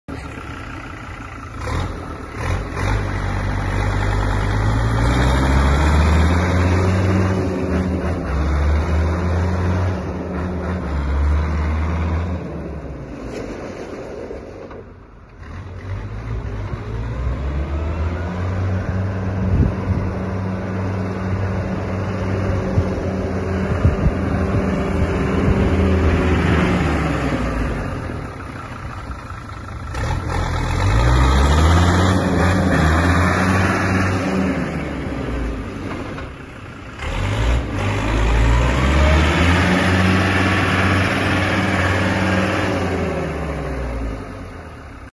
BV5721 Fiat 90-90
Fiat 90-90
4x4
Supercomfort cabin
Good tires
Tractor is in very good condition

Stock nr: BV5721